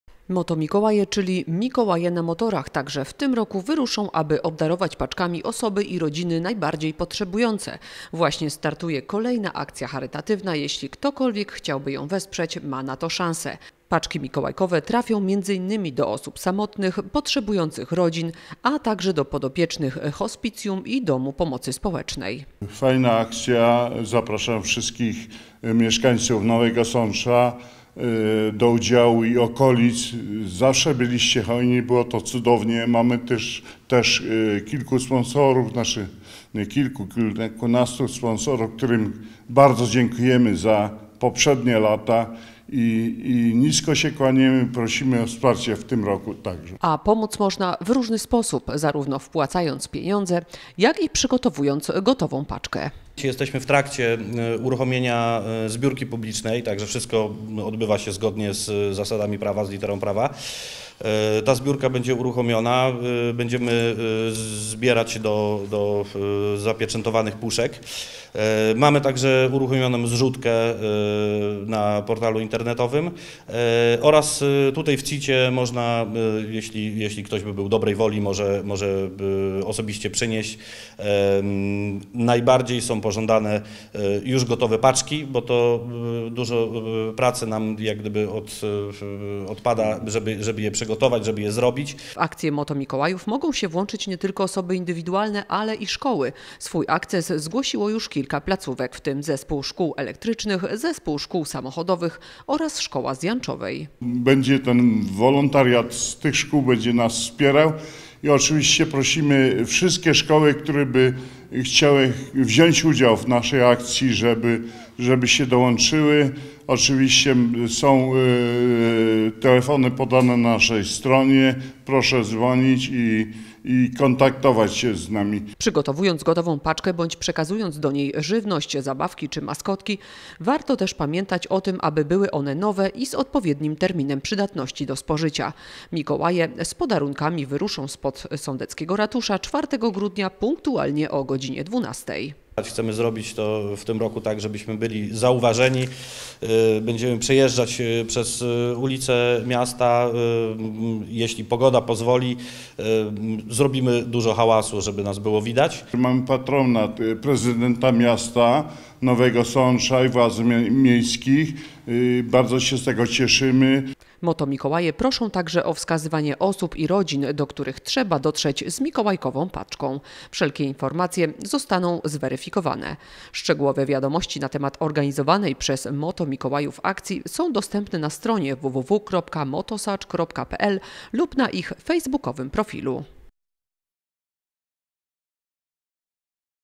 Rusza akcja Motomikołaje 2021. Każdy może pomóc
Tradycyjnie, jak co roku motocykliści z grupy MotoSącz będą rozwozić mikołajkowe paczki. Trafią one do osób samotnych, potrzebujących rodzin, pensjonariuszy Domów Pomocy Społecznej i hospicjum. W akcji może też pomóc każdy z nasz. Szczegóły w materiale filmowym.